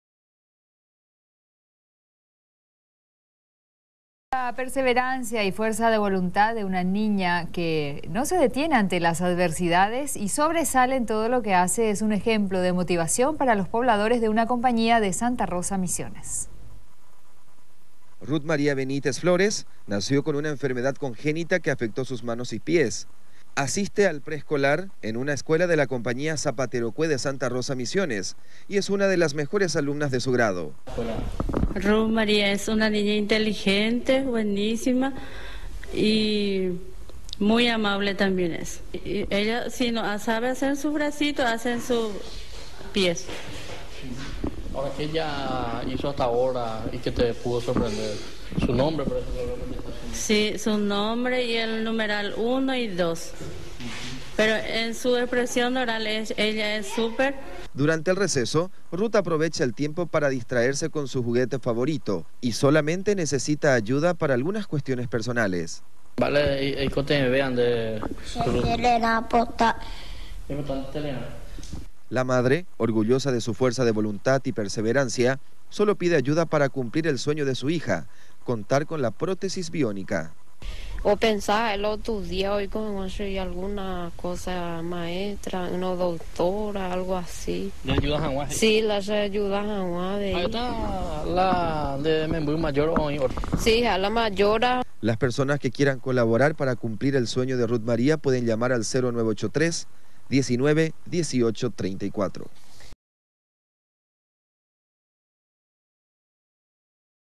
Niña nació sin manos y es ejemplo de tenacidad - 05/06/14
Niña de Santa Rosa - Misiones nació sin manos y con problemas en los pies. Esto no le impide ser una de las mejores alumnas de su escuela. Sueña con llegar a tener manos biónicas. Madre pide ayuda al Gobierno para que se cumpla esto.